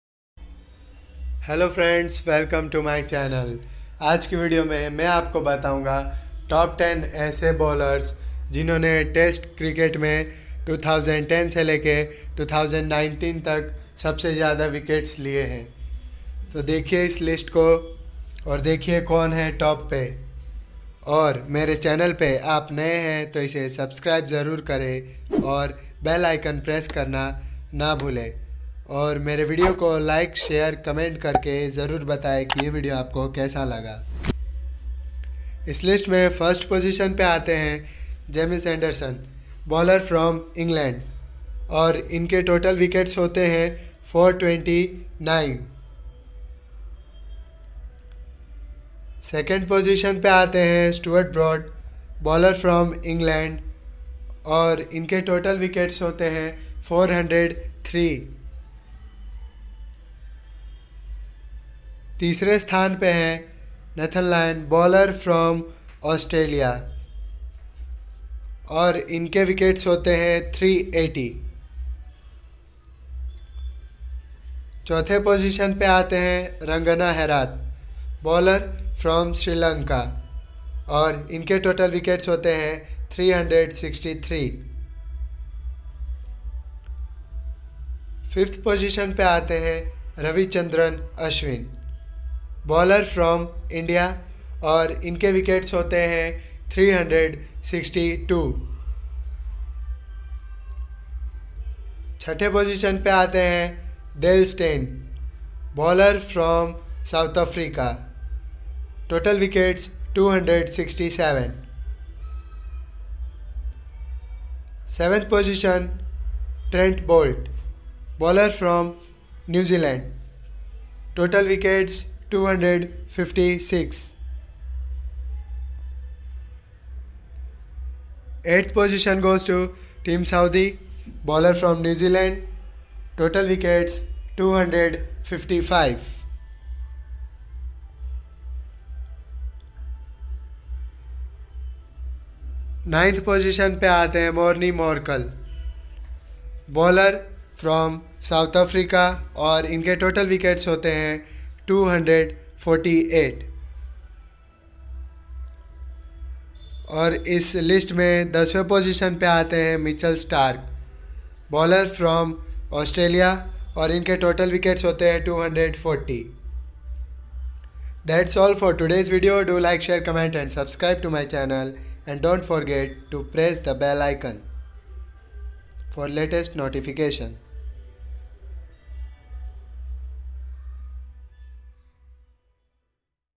Top 10 bowlers with maximum wickets in test cricket between 2010-2019
Full list of top 10 bowlers with maximum number of wickets in test cricket between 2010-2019